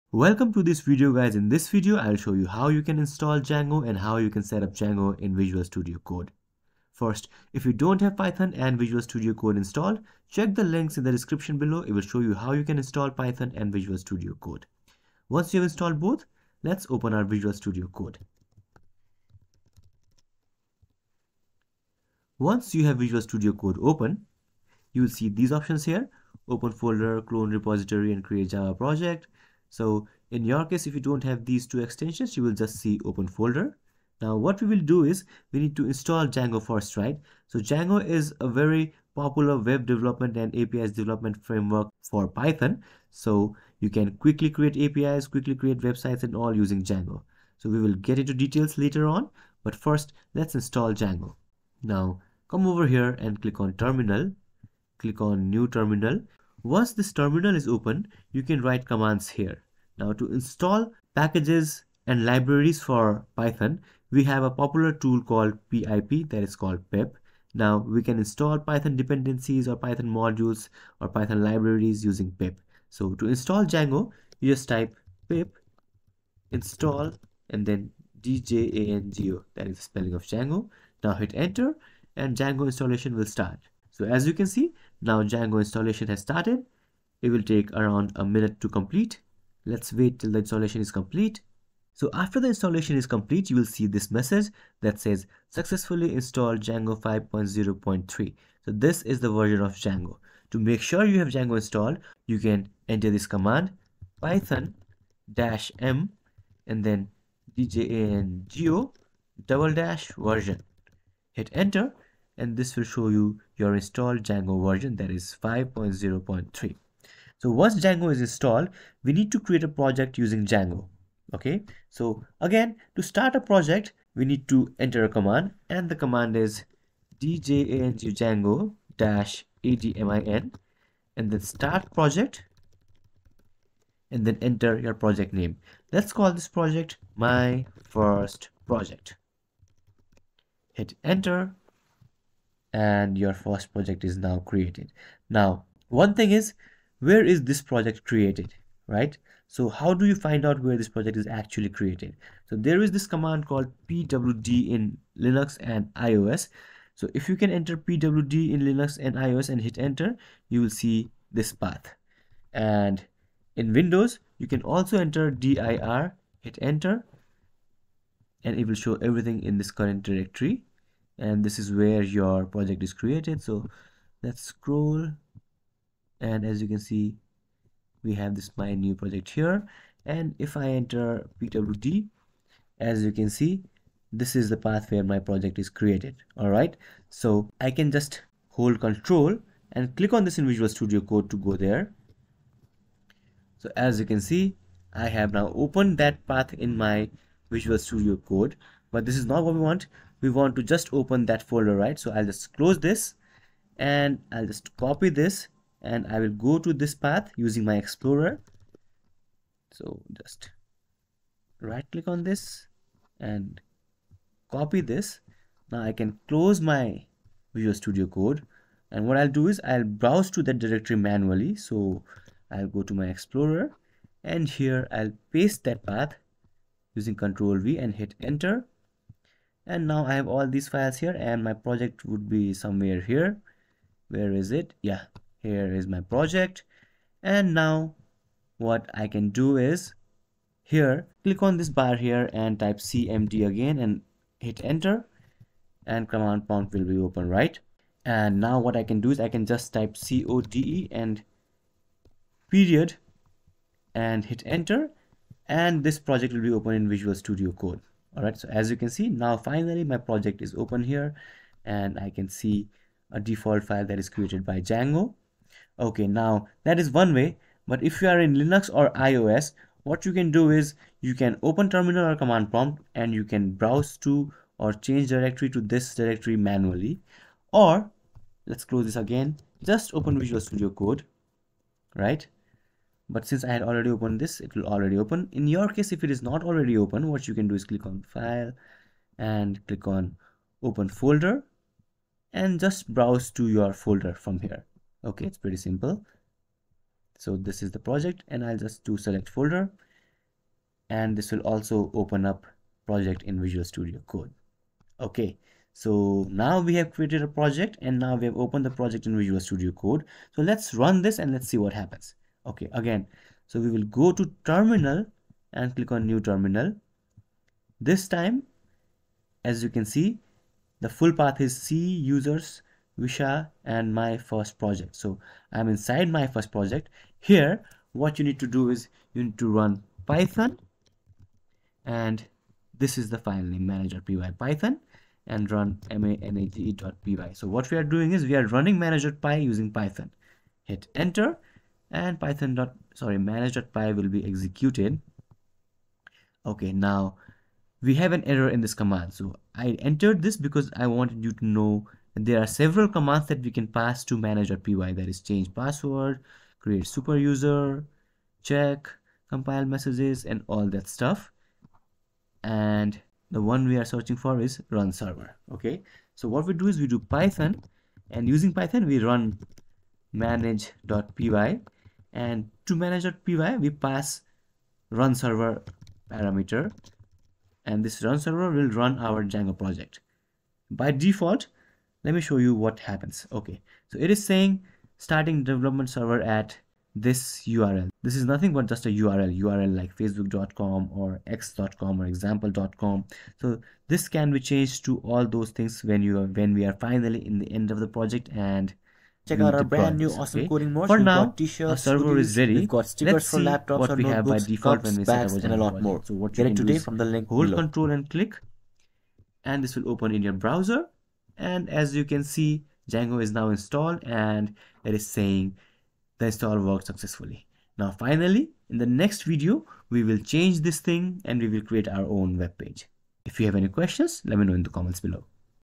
How to Install & Run Django 5.0.3 in Visual Studio Code (2024)
In this video, I'll show you how you can Install Django 5.0.3 in visual studio code (vscode) and how you can run django in vscode.
Django is a very popular open-source python framework for web development.

By the end of this video, we will have install and setup Django in Visual studio. We will also see various methods of how we can open a django project in visual studio code(vscode)